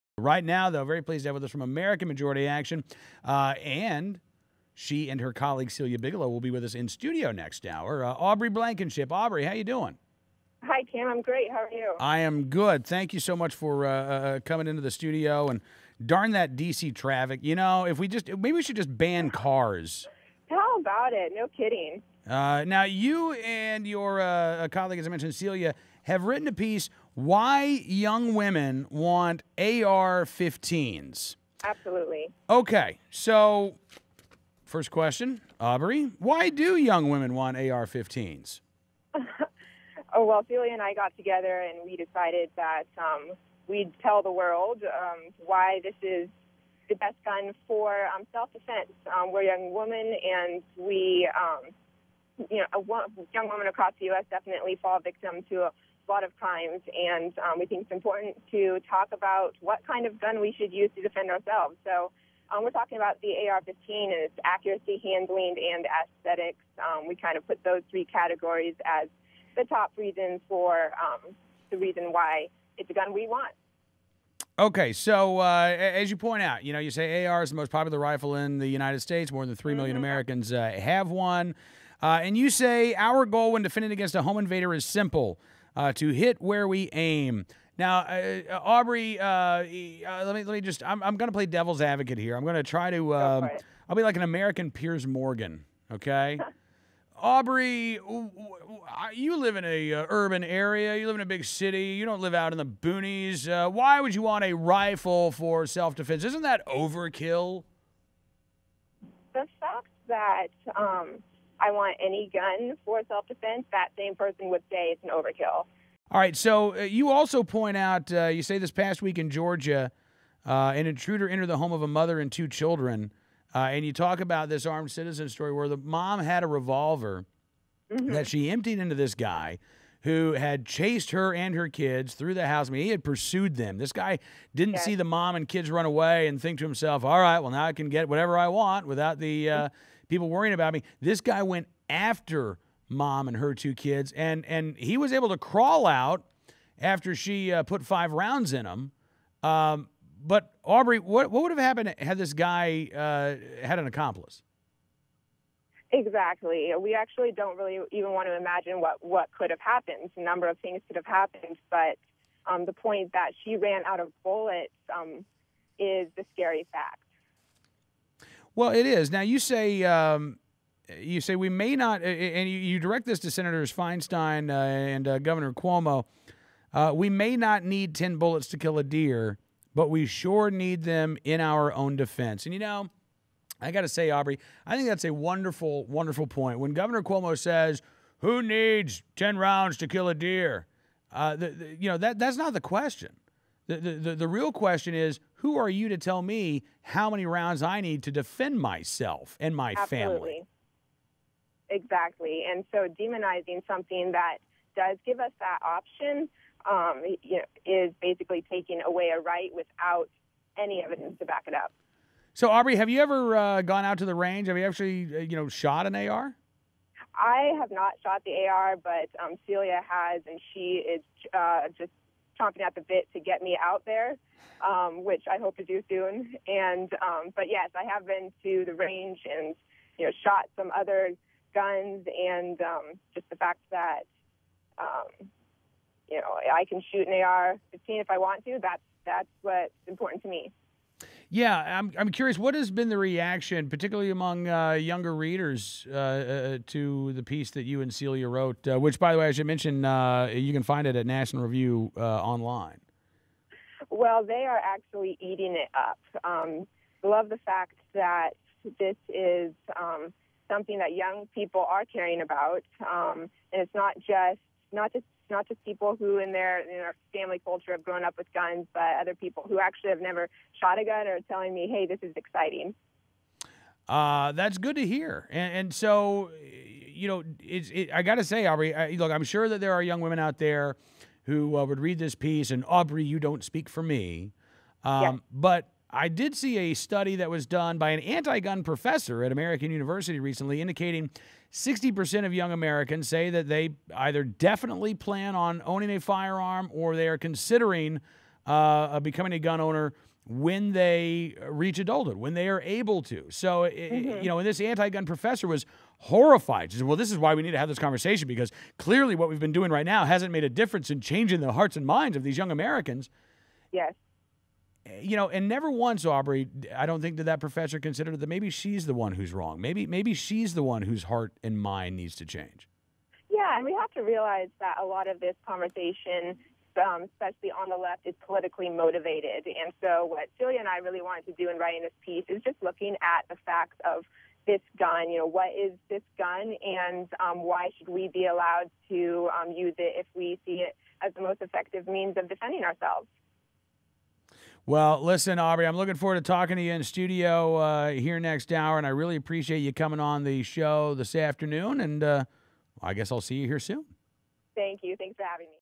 Why Young Women Want AR-15s
Cam Edwards talks to Aubrey Blankenship from American Majority Action, who wrote this article with Celia Bigelow for National Review - NRA News - January 23, 2013